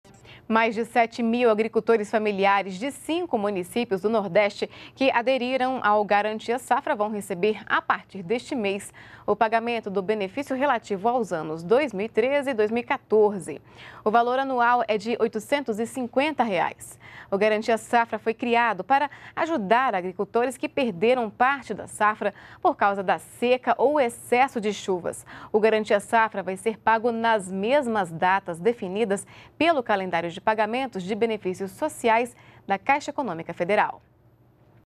Agricultores do semiárido vão receber pagamento do Garantia-Safra
NBR NOTÍCIAS - 17.06.15: Foi publicado nesta quarta-feira (17), no Diário Oficial da União (DOU), a autorização do Ministério do Desenvolvimento Agrário (MDA) para o pagamento dos benefícios relativos à safra 2013/2014 a agricultores que aderiram ao Garantia-Safra. No total, 7.118 agricultores de estados do Nordeste receberão o benefício.